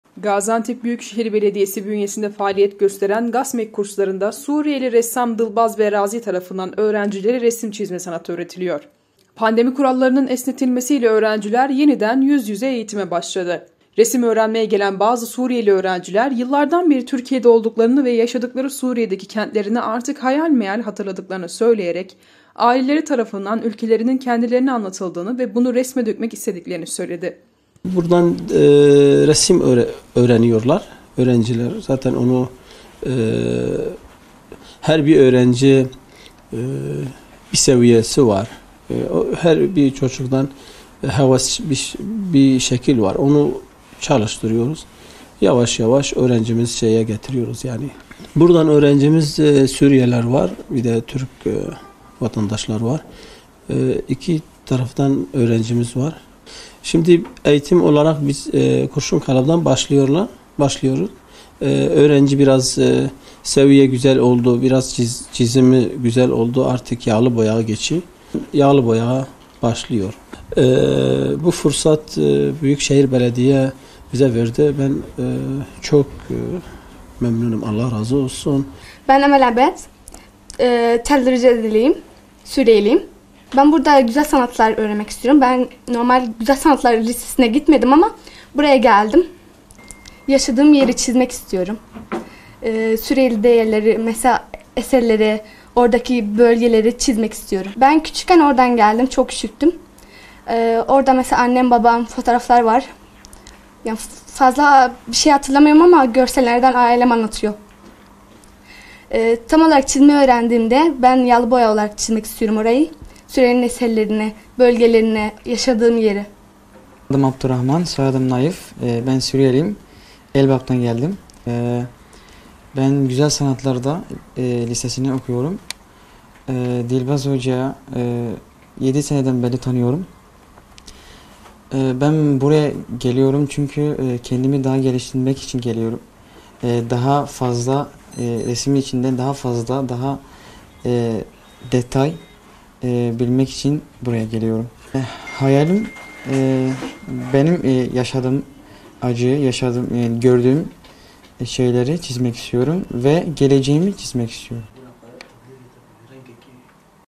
HAYALLERİNDE YAŞATTIKLARI ÜLKELERİNİ ÇİZECEKLER
Gaziantep Büyükşehir Belediyesi bünyesinde faaliyet gösteren GASMEK kurslarında Suriyeli Ressam Dılbaz Berazi, tarafından öğrencilere resim çizme sanatı öğretiliyor. Pandemi kurallarının esnetilmesiyle öğrenciler yeniden yüz yüze eğitime başladı. Resim öğrenmeye gelen bazı Suriyeli öğrenciler yıllardan beri Türkiye’de olduklarını ve yaşadıkları Suriye’deki kentlerini artık hayal meyal hatırladıklarını söyleyerek, aileleri tarafından ülkelerinin kendilerine anlatıldığını ve bunu resme dökmek istediklerini söyledi.